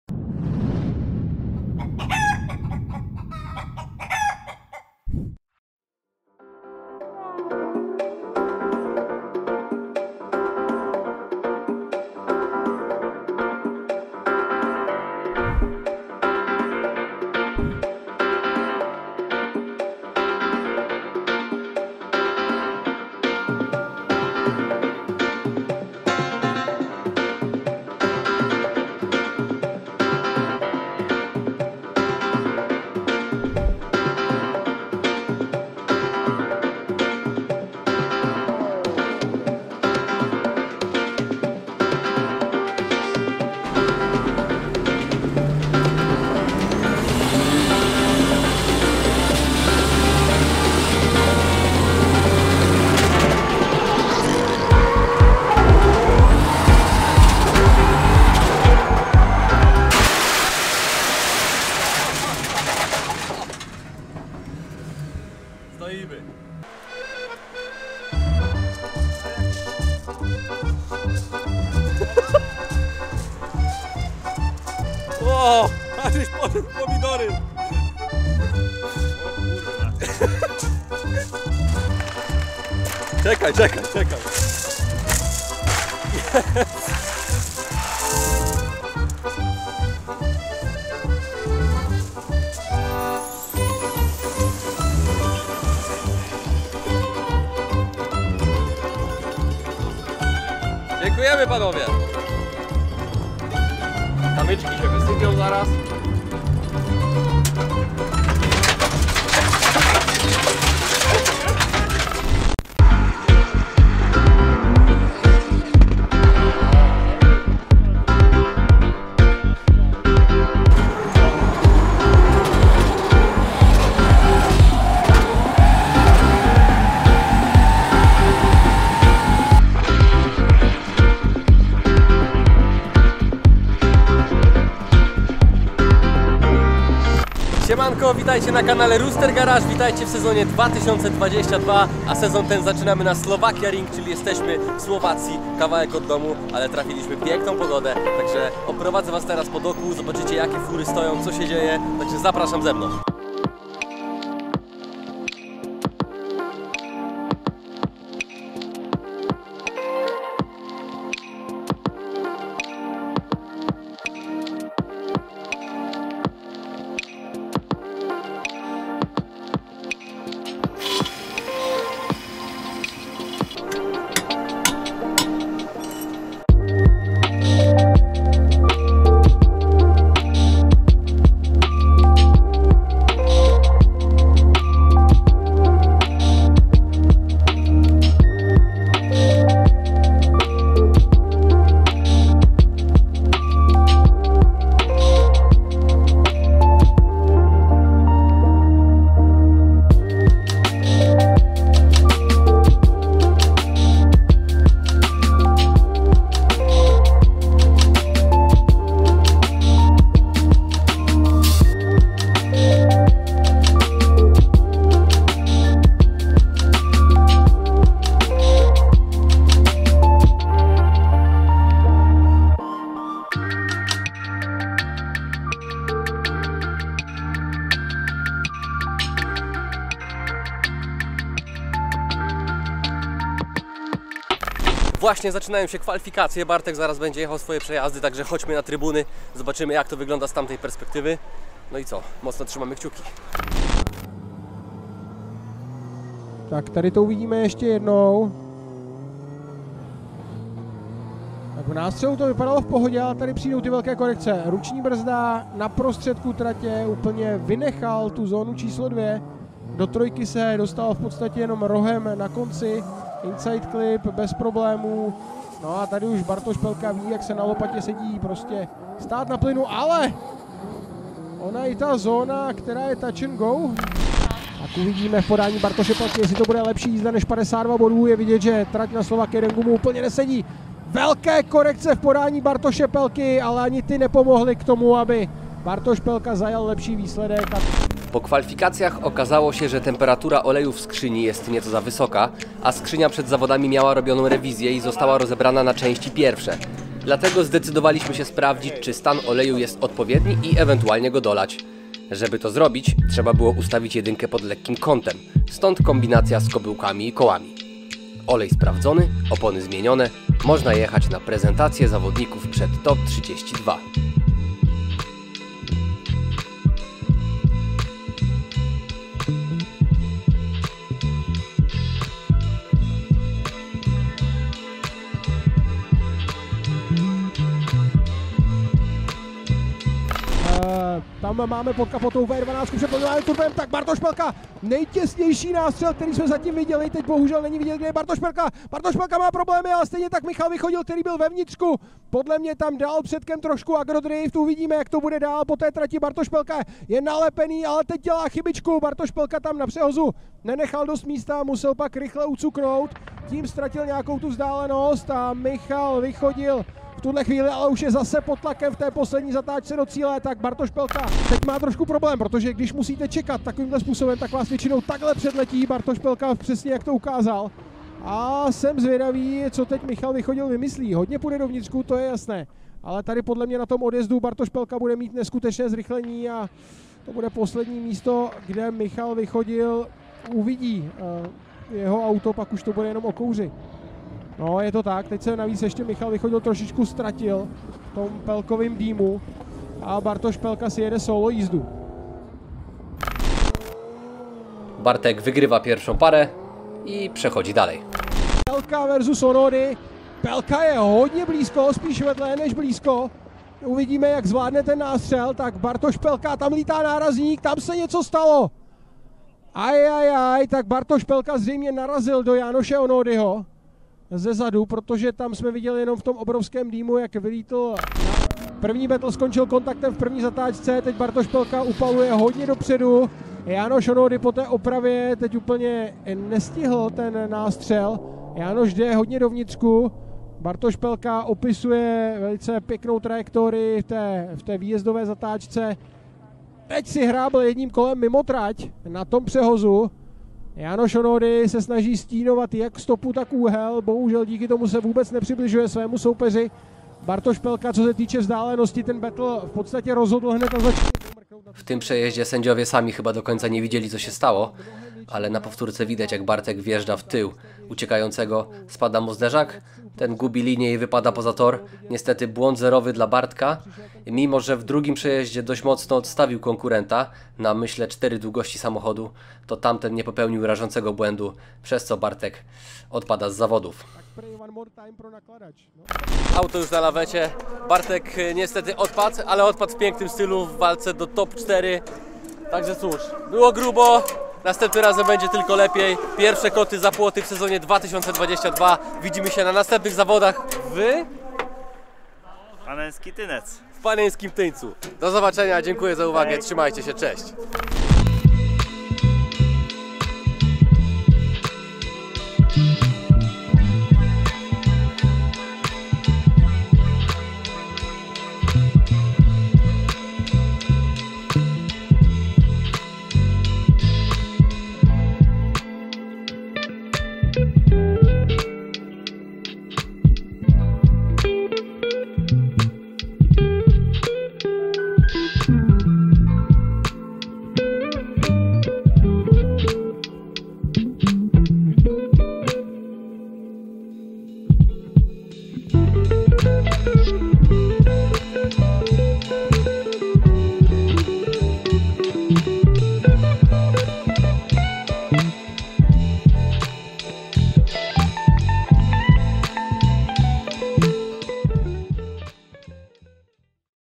SLOVAKIARING 2022 RD1 Transportprojekt Drift Challenge | Bartosz Pełka | Dziennik Rooster Garage
Witamy na kanale Rooster Garage!

Zapraszamy do odcinka z serii Dziennik Rooster Garage, w którym zobaczycie wyjazd na pierwsze w sezonie 2022 zawody CDS na Slovakiaring, które odbyły się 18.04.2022.

Dziękujemy i życzymy miłego oglądania.

Skrzynia 8HP ze sterownikiem TCU TurboLamik w ustawieniach "Drift" z wirtualnym sprzęgłem na potencjometrze.

Samochód: BMW E82 z silnikiem M73B44 V12 Turbo o mocy 800 KM i 1100 NM.